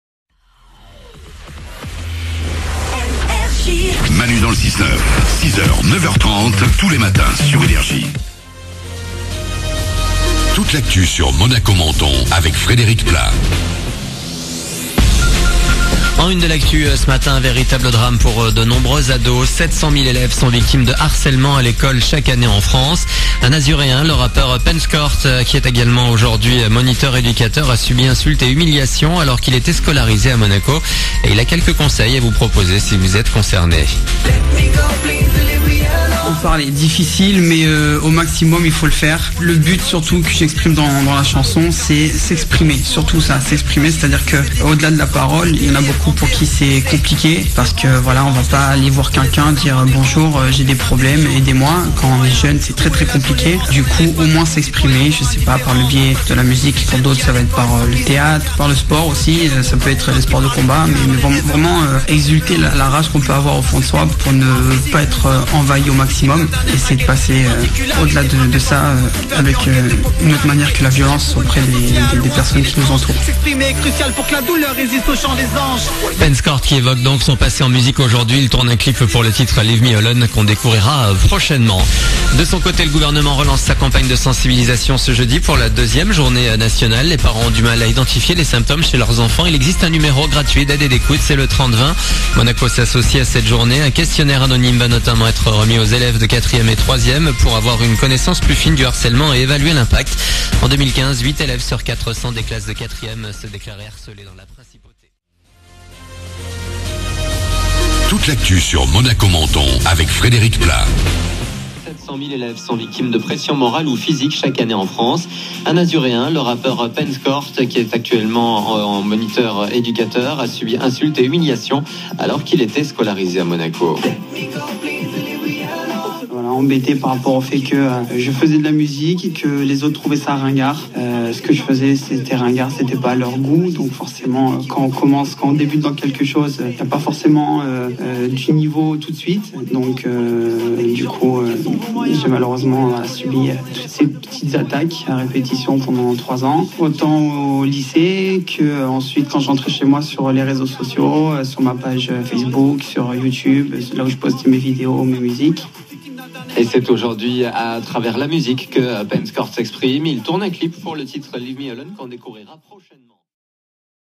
Penn Skort - Passage Radio "NRJ Monaco/Menton"
Voici un petit rattrapage pour celles et ceux qui n'ont pas pu écouter l'interview réalisée sur la radio "NRJ Monaco/Menton", à propos de mon projet de clip contre le harcèlement scolaire.

Merci à Frédéric PLAS, journaliste NRJ, pour l'intérêt porté à ce projet !